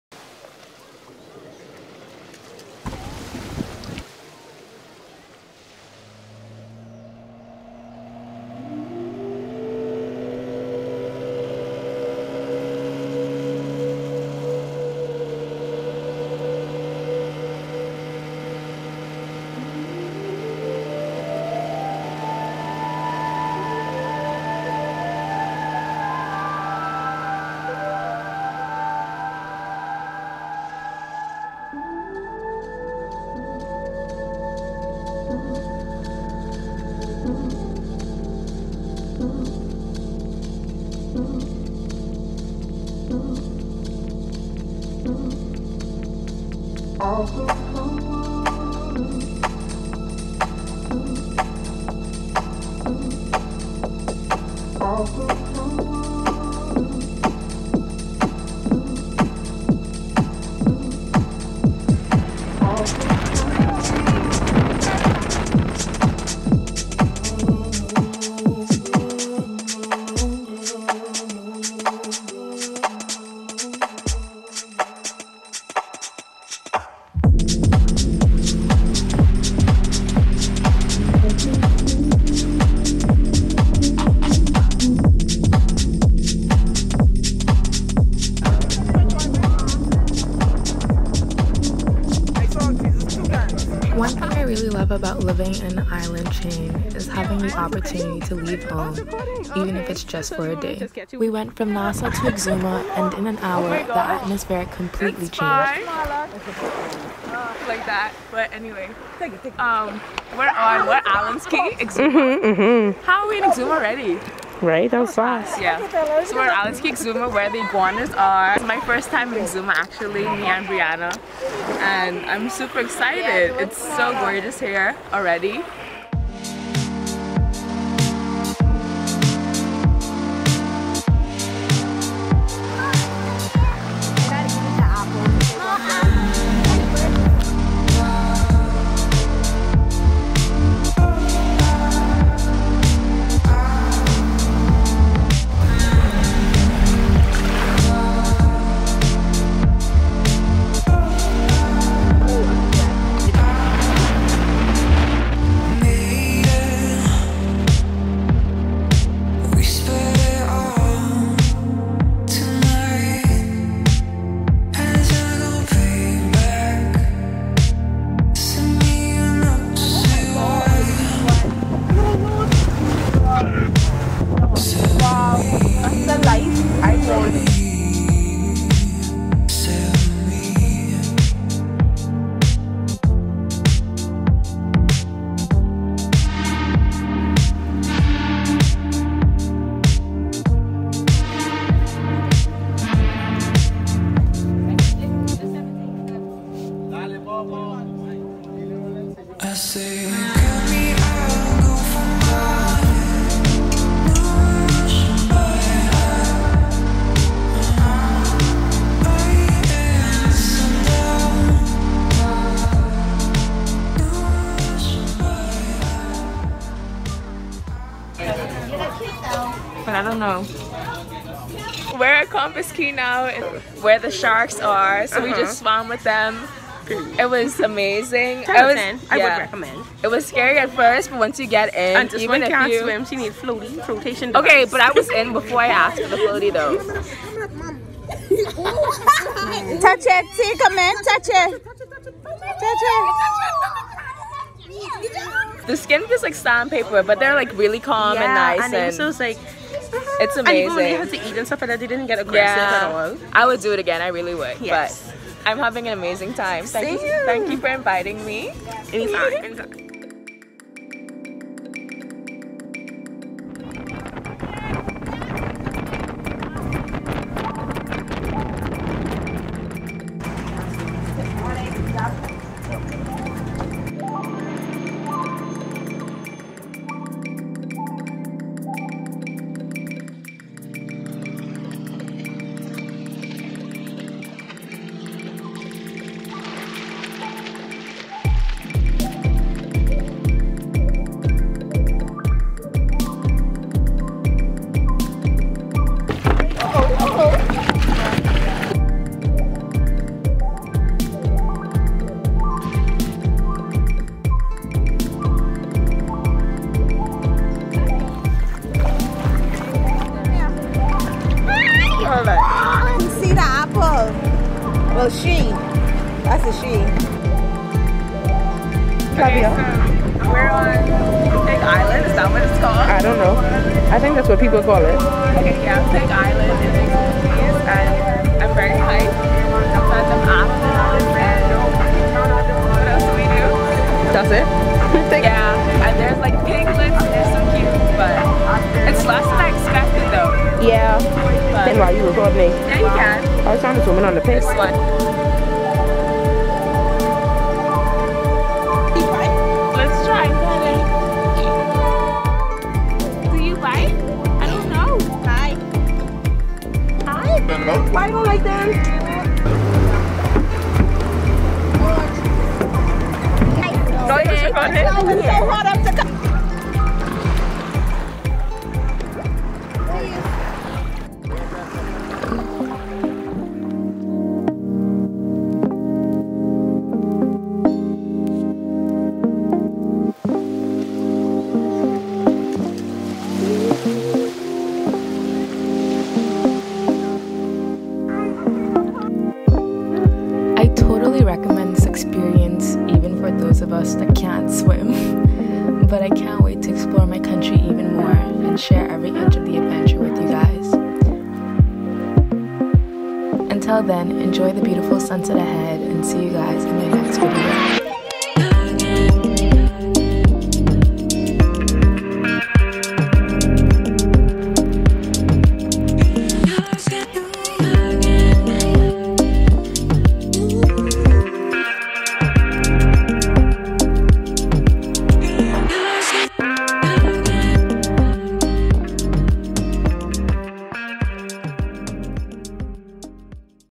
EXUMA | Day Trip Vlog (Swimming Pigs, Petting Shark, Feeding Iguanas)
Welcome to one of the other 700 islands and cays! On random Tuesday my friend (Brinna) and I had a little boat trip and I decided to casually vlog for you guys, nothing to fancy. I hope you enjoy and let me know which island you'd like to see me visit next. Enjoy! XOXO.